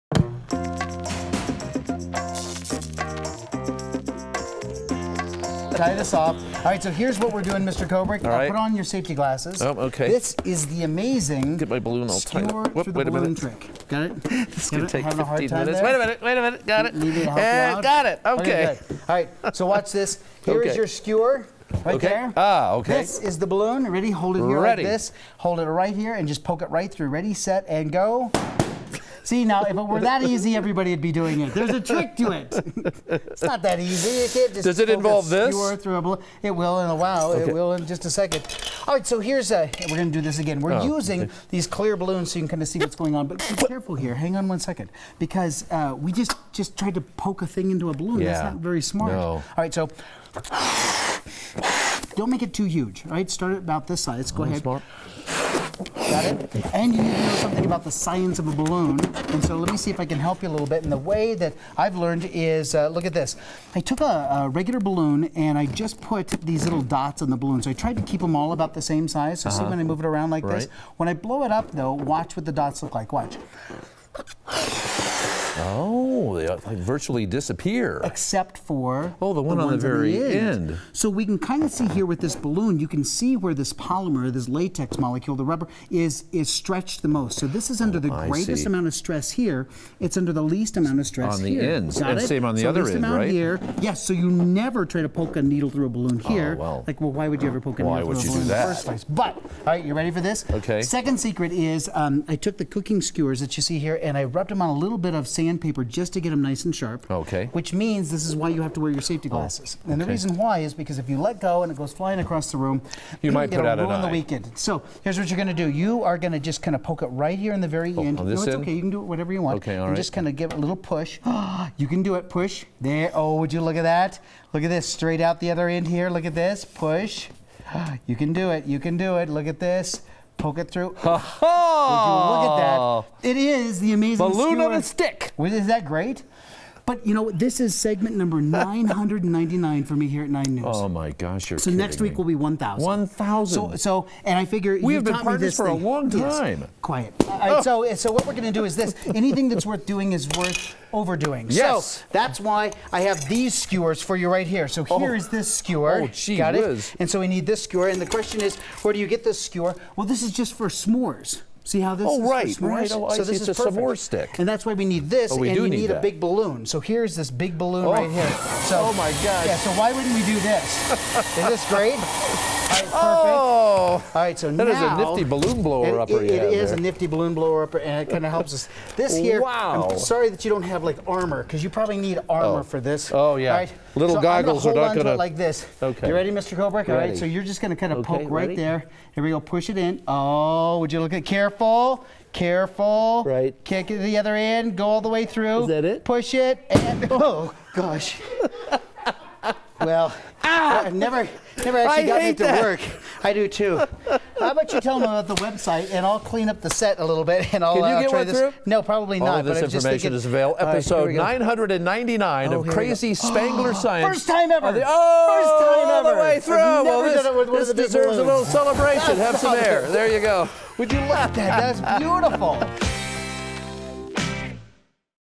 Balloon Science - How to Push a Skewer Through a Balloon - Cool Science Experiment
It's a Spangler Science Monday and Steve has traded in his fire extinguisher for balloons and cooking skewers. It's a lesson in the science of polymers… but something is bound to explode.